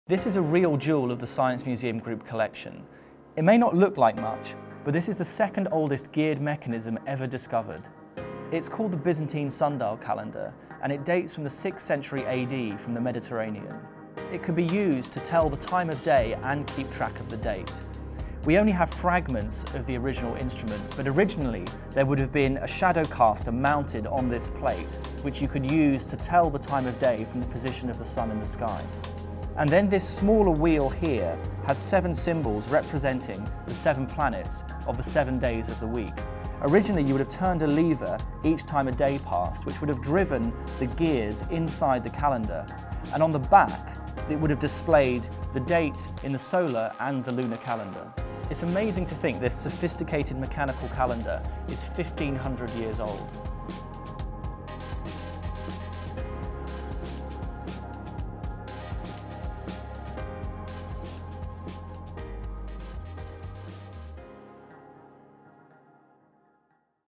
The Byzantine sundial: telling the time in the 6th century
This ancient mechanical calendar is the second oldest geared device ever discovered.

It included both a sundial for telling the time and a geared calendar, driven by a dial showing the seven days of the week. Although only these fragments survive, evidence suggests that it may have been used to convert between a popular calendar based on the phases of the Moon and the official Sun-based calendar of the Byzantine Empire.

See this sundial calendar in our blockbuster exhibition:

The Sun: Living With Our Star
6 October 2018 – 6 May 2019